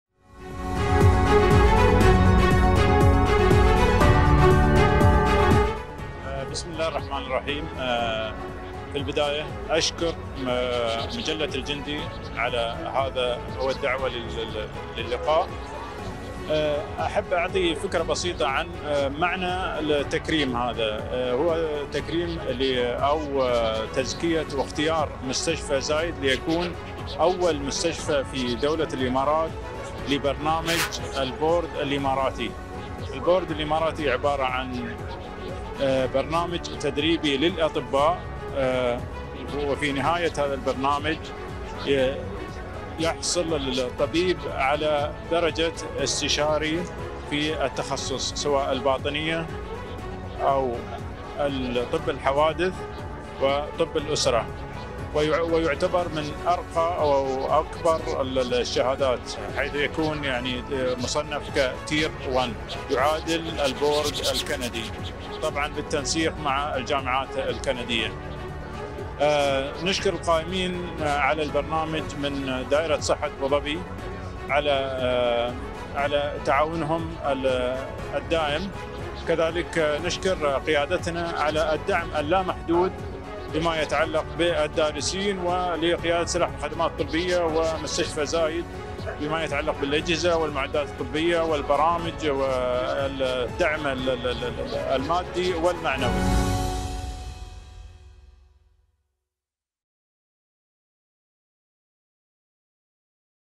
العميد الركن الطبيب سرحان محمد سعيد النيادي، قائد سلاح الخدمات الطبية بالوكالة
مجلة_الجندي تلتقي العميد الركن الطبيب سرحان محمد سعيد النيادي، قائد سلاح الخدمات الطبية بالوكالة، بمناسبة تكريم واعتماد مستشفى زايد العسكري - سلاح الخدمات الطبية، كأول مؤسسة صحية في دولة الإمارات تحصل على الاعتماد المؤسسي من قبل المعهد الوطني للتخصصات الصحية (البورد الإماراتي)